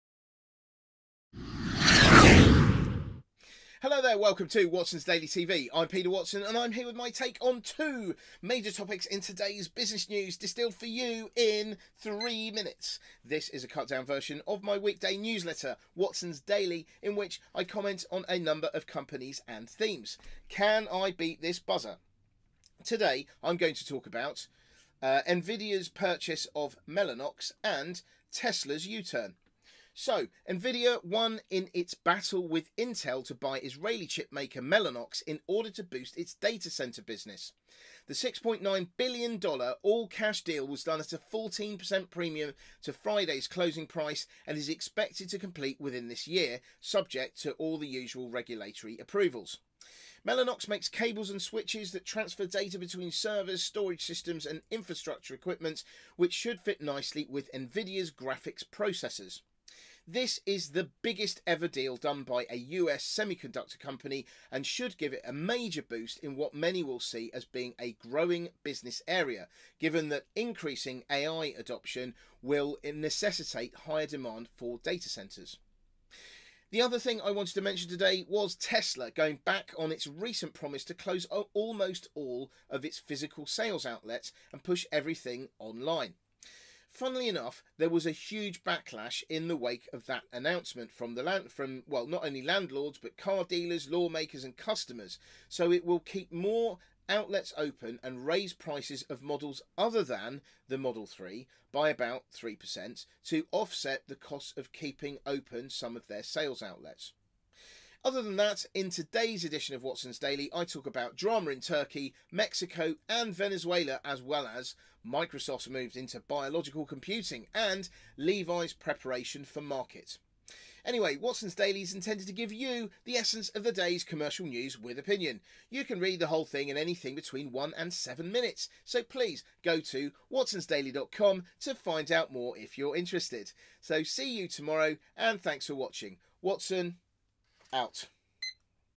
Watson’s Daily TV, Tuesday 12th March 2019
In today's edition, I talk about Nvidia's purchase of Mellanox and Tesla's U-turn